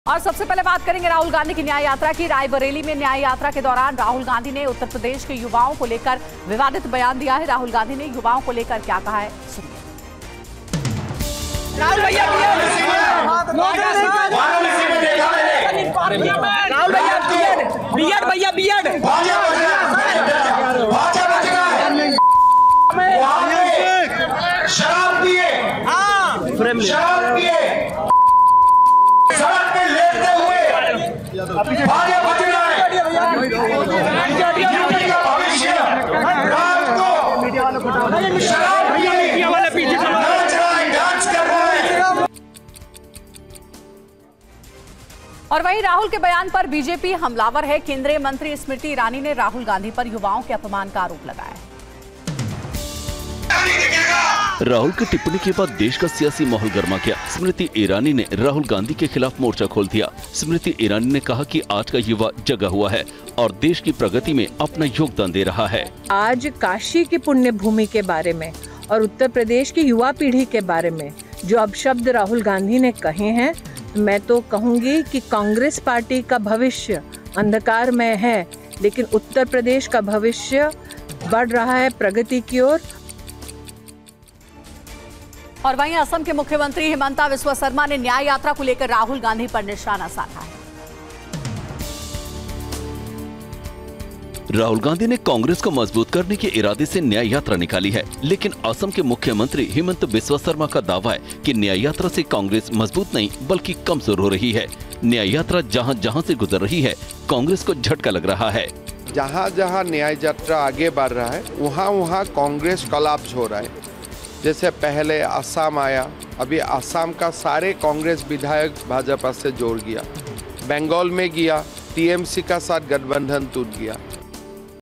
Nyay Yatra: Rahul Gandhi ने Uttar Pradesh के युवाओं को लेकर क्या कहा? | Congress | BJP
रायबरेली में न्याय यात्रा के दौरान राहुल गांधी ने उत्तरप्रदेश के युवाओं को लेकर जो बयान दिया है उसपर सियासत गरमा गई है..राहुल गांधी ने युवाओं को लेकर क्या कहा...आपको सुनाते हैं..